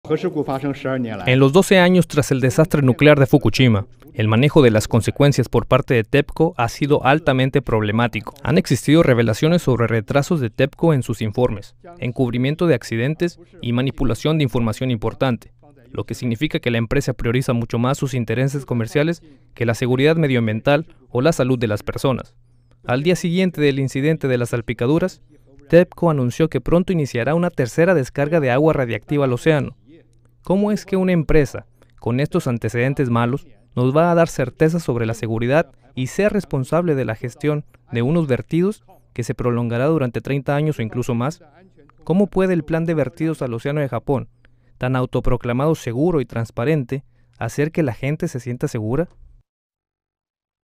Dos hombres fueron hospitalizados después de ser salpicados por el líquido contaminado en Japón
El Ministerio de Relaciones Exteriores de China ha acusado de engaño a la compañía encargada de la accidentada central nuclear de Fukushima. La semana pasada, dos trabajadores fueron hospitalizados después de ser salpicados por el agua radiactiva. En un inicio, la compañía TEPCO afirmó que el incidente había involucrado 100 mililitros de aguas residuales. Más tarde, modificaron la cantidad por "varios litros". La cancillería china también dijo que el incidente expone nuevamente los problemas de antaño de la administración interna de TEPCO y su costumbre de engañar a la opinión pública.